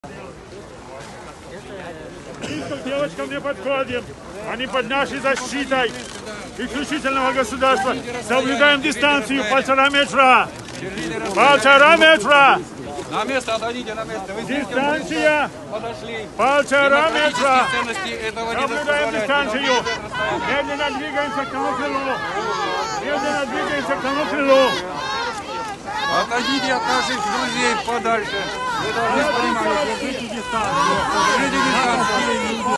Провокаторы на акции в Москве начали раздавать доллары, разговаривая на «ломанном русском».
На Арбат подтянулись провокаторы из НОД. В касках NATO они начали раздавать доллары, разговаривая на «ломанном русском».